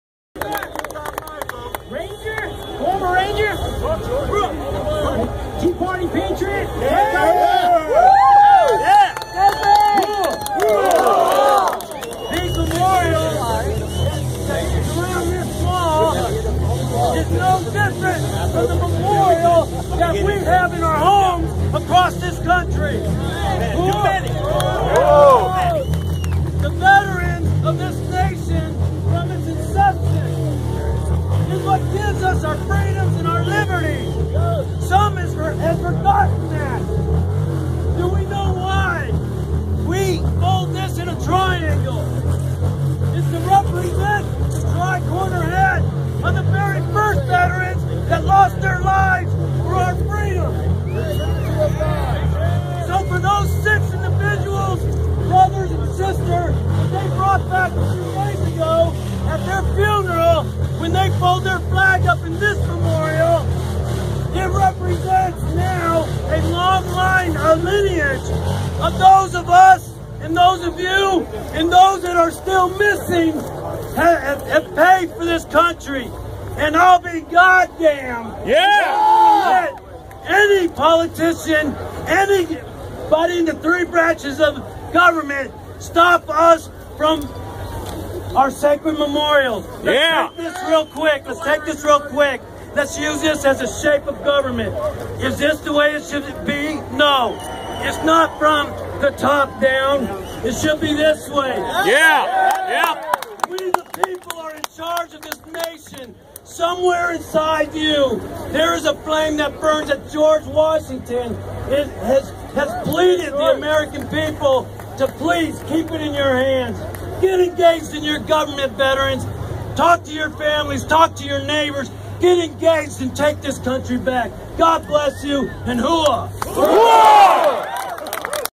Million Vet March takes WWII Barricades to the White House. Memorial of the Flag.